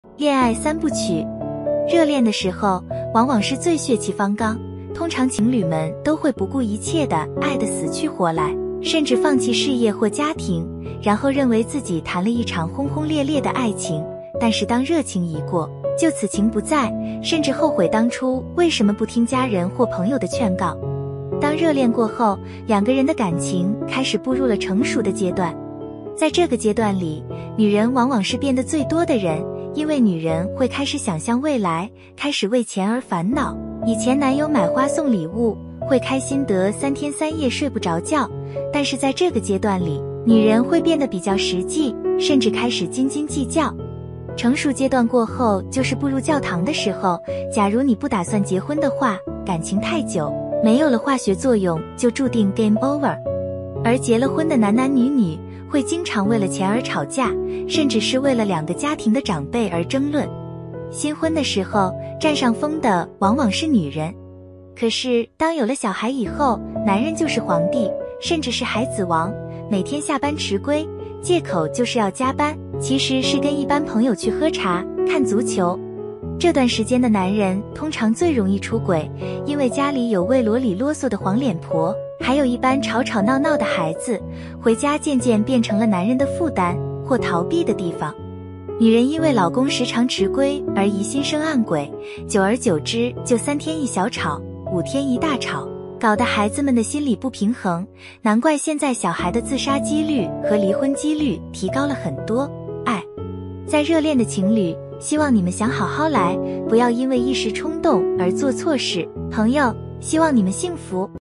恋爱三部曲
热恋的时候， 往往是最血气方刚， 也就是最容易做错事的时候， 通常情侣们都会不顾一切地爱得死去活来， 甚至放弃事业或家庭， 然后认为自己谈了一场轰轰烈烈的爱情